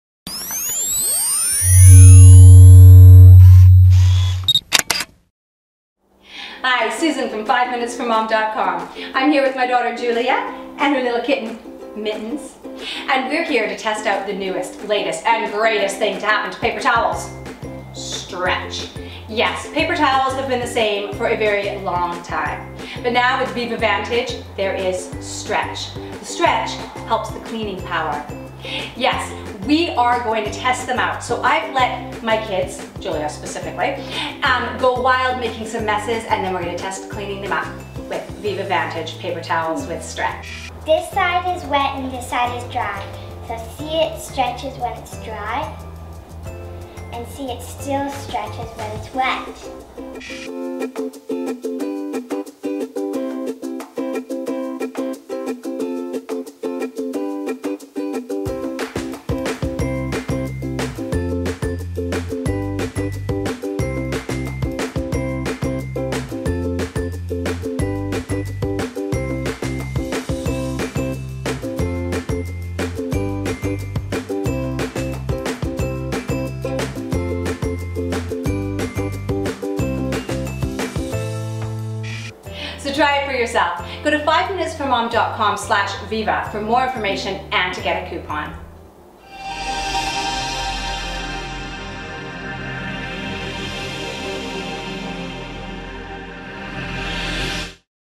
Viva Vantage Paper Towel Review - Testing the Stretch
- Susan and her daughter Julia, along with their kitten "Mittens", test out new Viva Vantage paper towels to see if they really stretch and how well they clean.

Follow 5 Minutes For Mom on